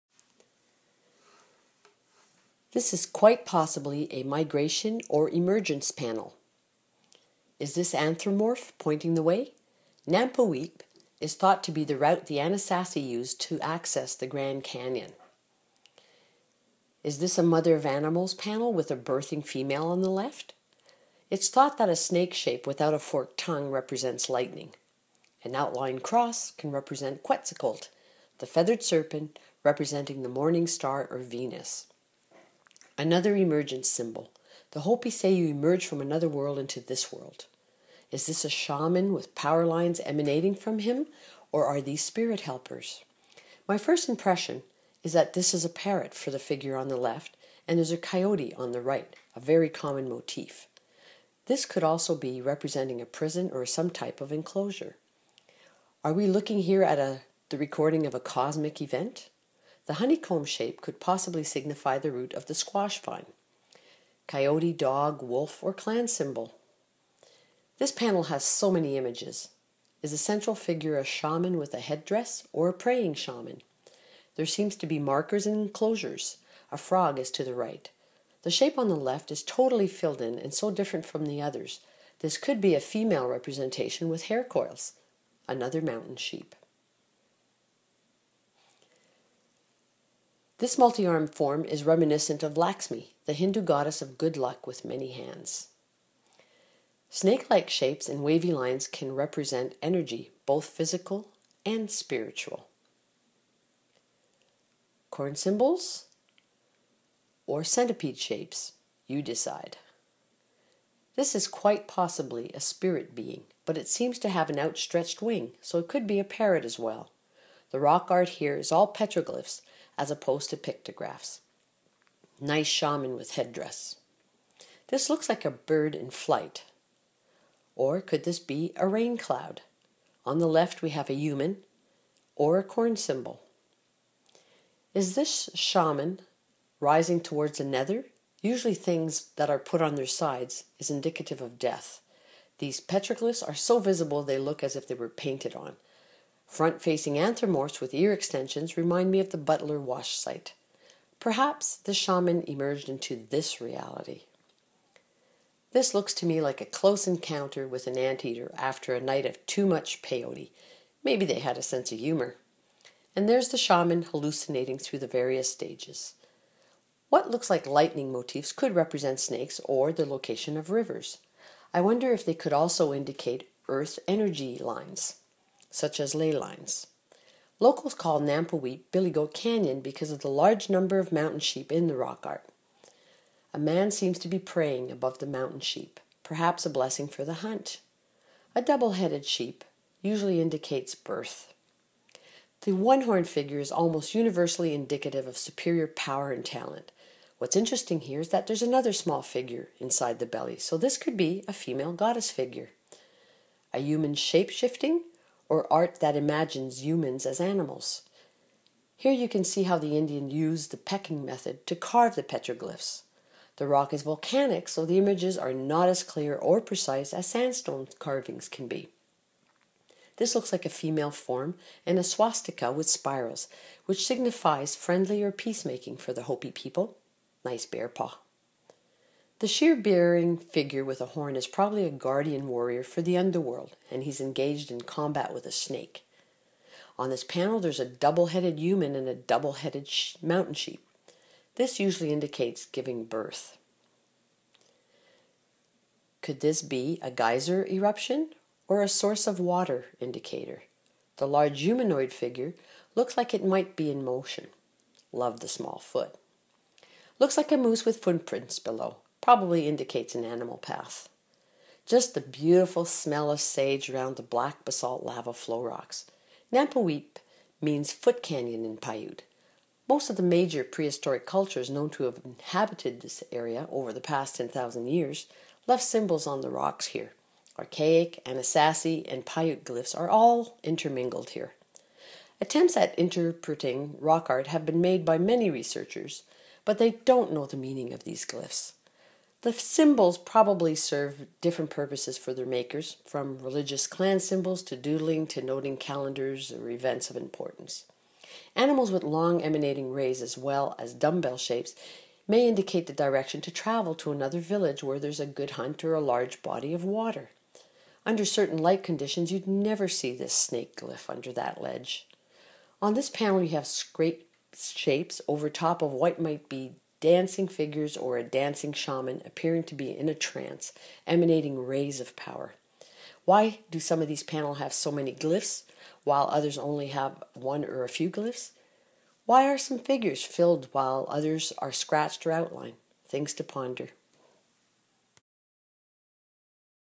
Petroglyphs at Nampaweap: Teyjah Explores Caves deep in the Arizona Strip
Nampaweap is a two-hour hike, moderate. Located deep in the Arizona Strip south of St. George, these two isolated sites represent very different cultures and artistic techniques.

Travelling south on a good, sometimes washboard, dirt road, you will stop at the Bundyville Schoolhouse for a rest break, then head up the Hurricane Cliffs on another good road stopping at the Uinkaret Pueblo and Nixon Springs. From there it is a short distance to Nampaweap, an extensive petroglyph site located in a shallow basalt canyon.

It is about a two-hour walk to see the glyphs which may require some light scrambling to see all of them. Then you will descend Mt. Tribal and travel the Clayhole Road to Paiute Cave, a large cavern in the basalt containing well-preserved Paiute pictographs. From there you return to St. George. A full day of driving and exploration.